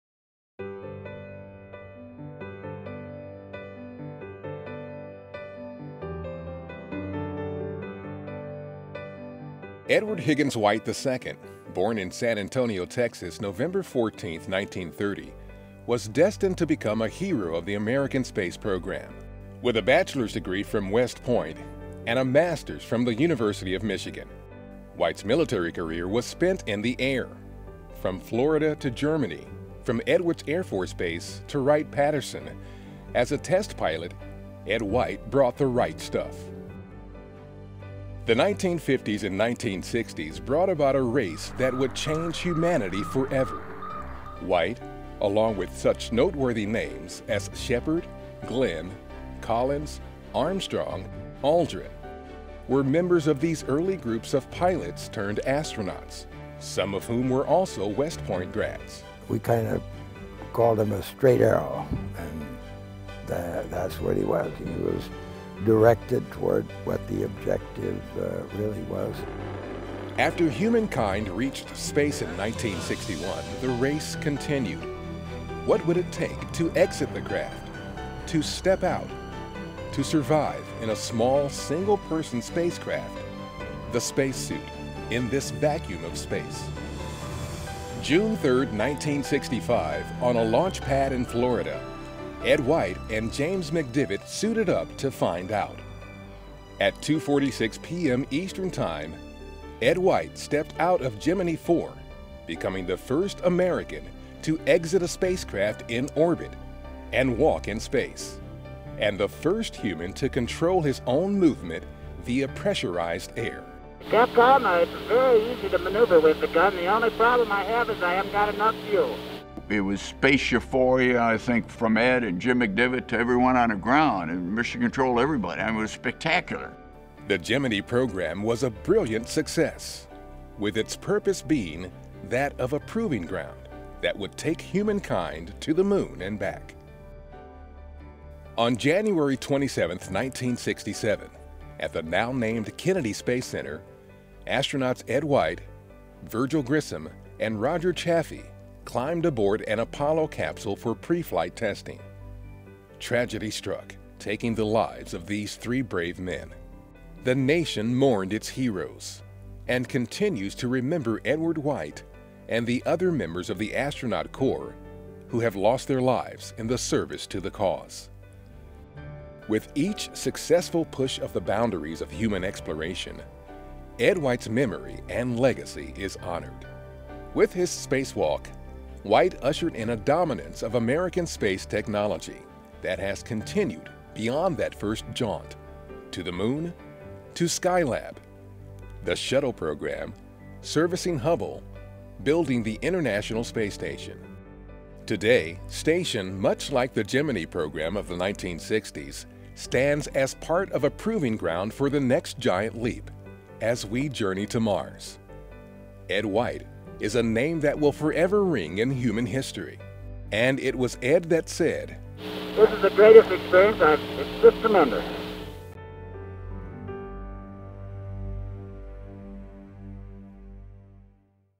Ed White Ambassador of Exploration
On June 3, 1965, when Gemini IV astronaut Ed White emerged from his spacecraft into the blackness of space, he became the first American to walk in space. For more than 20 minutes, White maneuvered himself around the Gemini spacecraft while it traveled from over Hawaii to the Gulf of Mexico--making his orbital stroll 6,500 miles long.